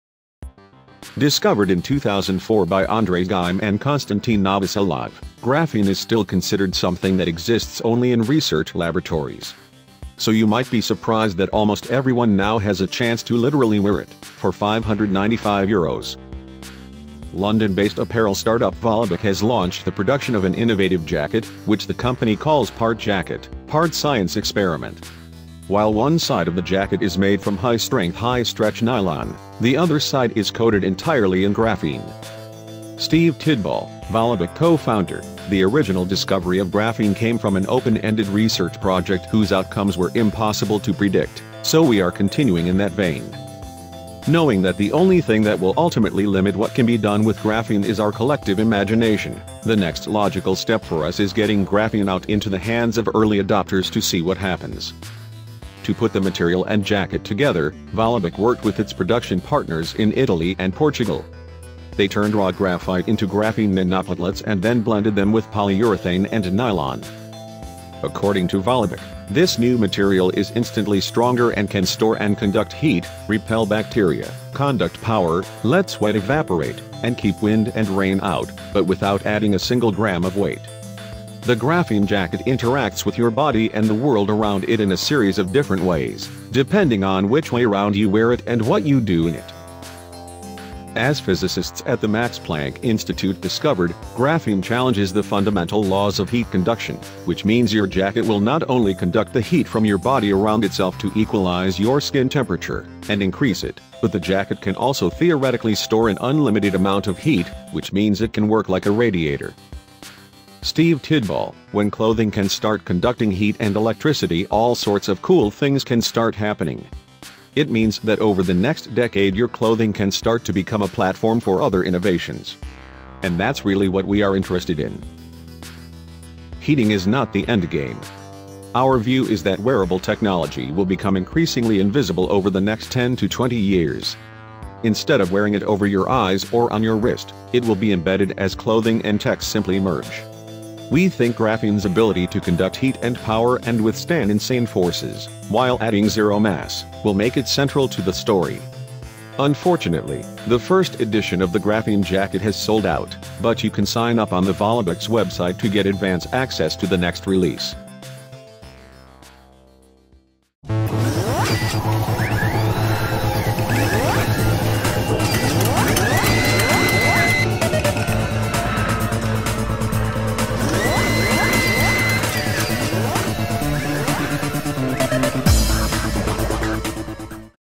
The World’s First Jacket Made with Graphene
discovered in 2004 by Andre Geim and Konstantin Novoselov, graphene is still considered something that exists only in research laboratories. So you might be surprised that almost everyone now has a chance to literally wear it,